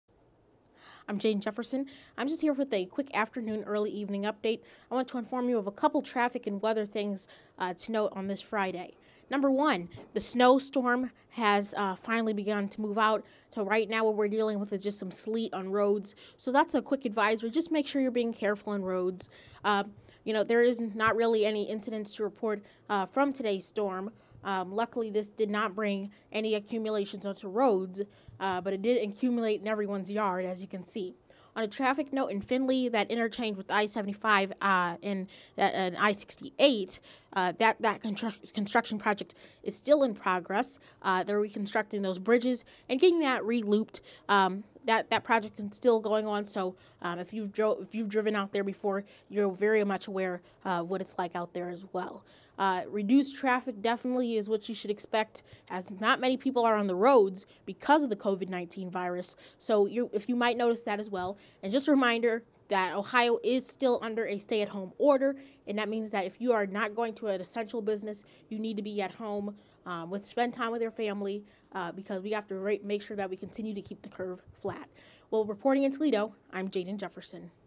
Afternoon Weather & Traffic Update with Jaden Jefferson
Here is your early-evening update. In Findlay, construction on the I-75/Route 68 interchange is still in progress, but nearing its end. Also, today's snowstorm should be moving out by 8pm.